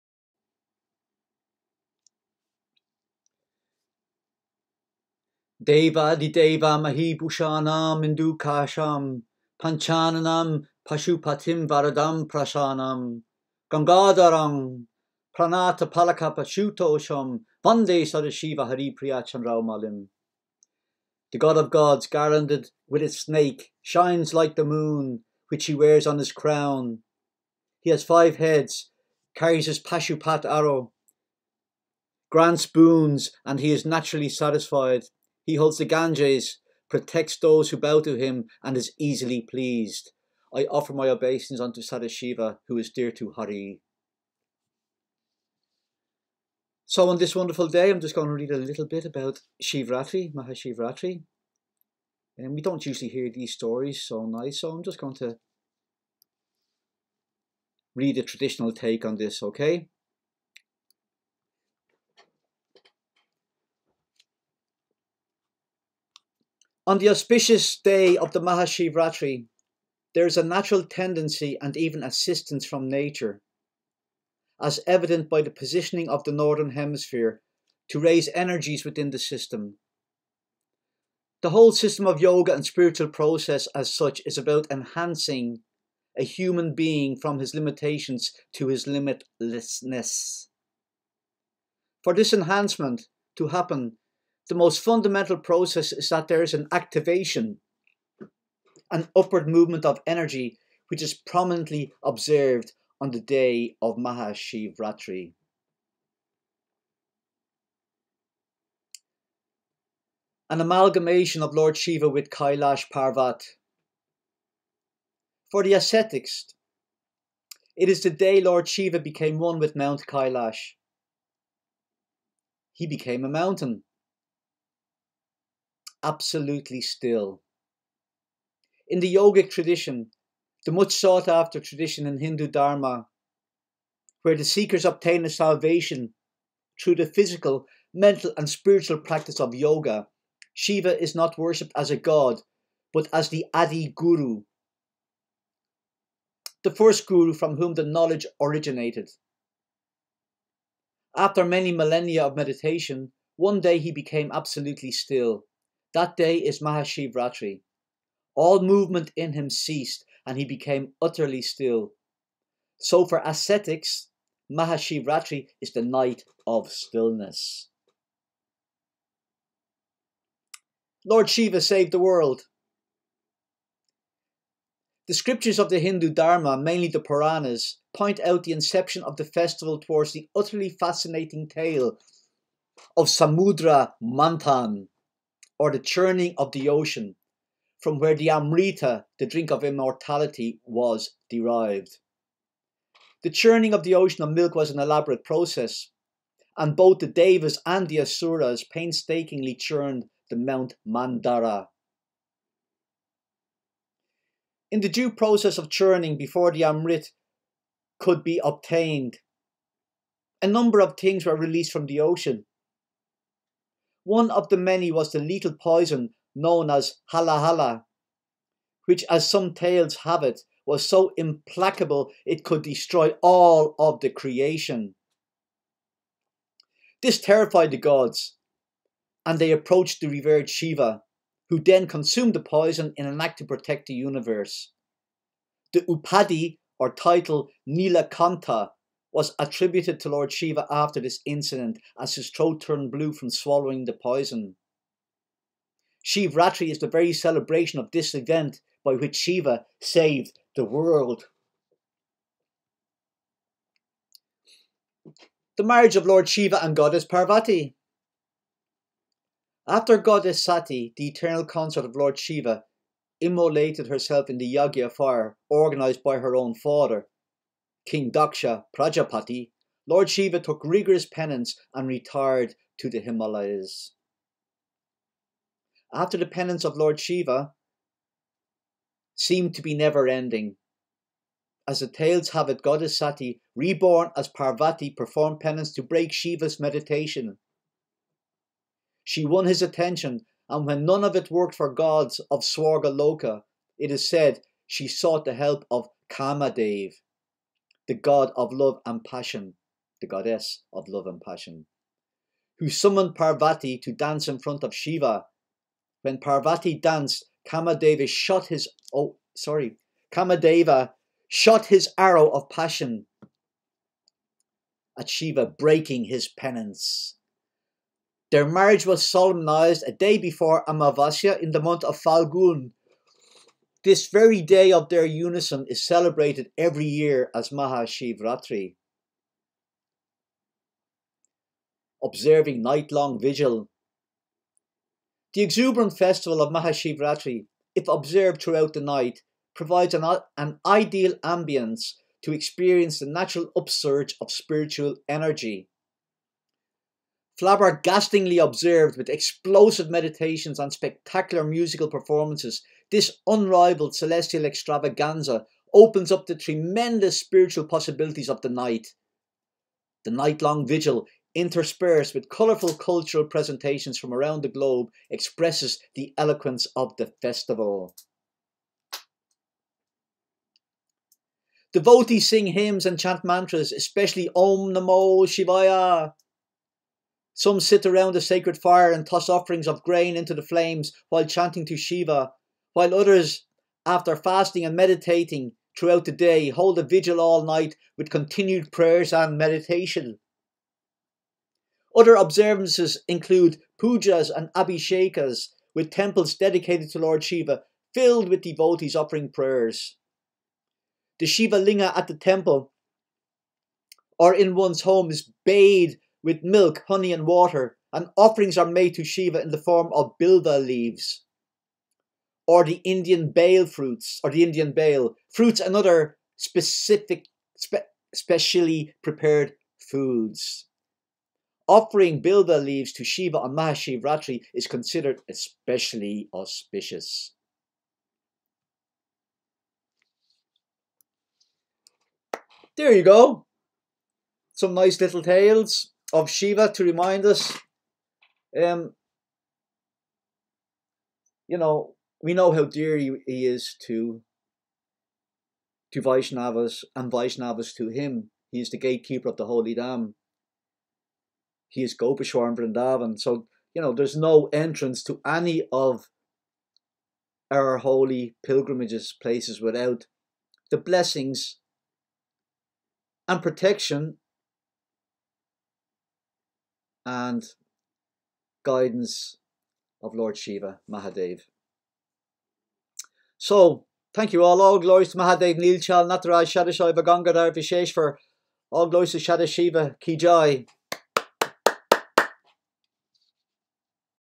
Maha Shivratri
Some pastimes of Lord Shiva to celebrate this auspicious day